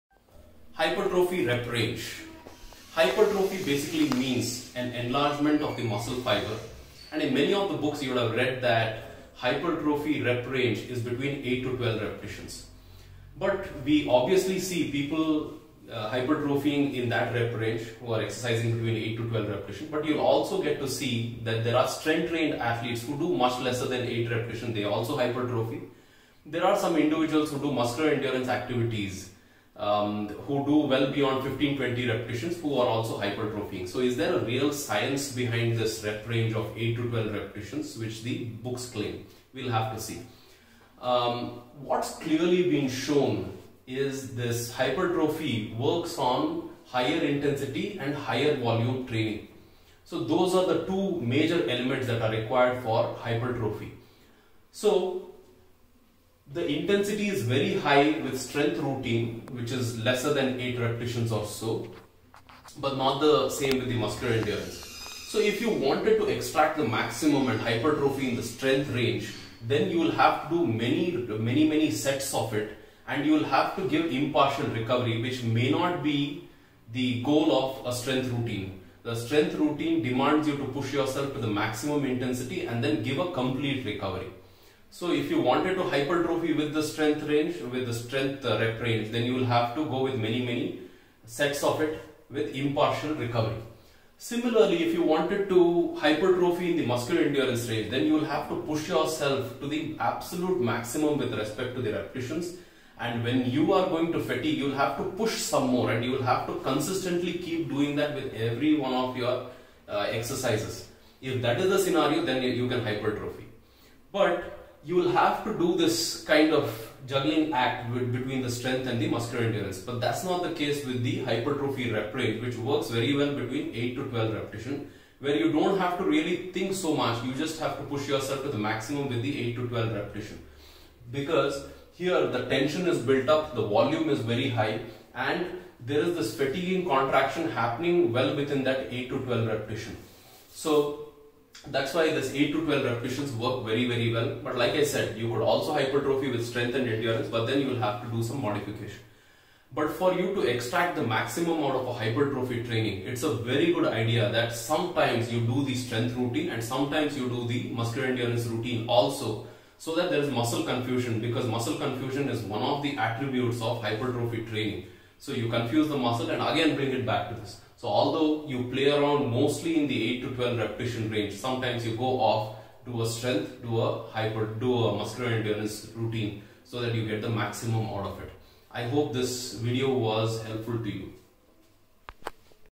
HYPERTROPHY REP RANGE
classic rep range for hypertrophy is 8 to 12 reps, but is it true?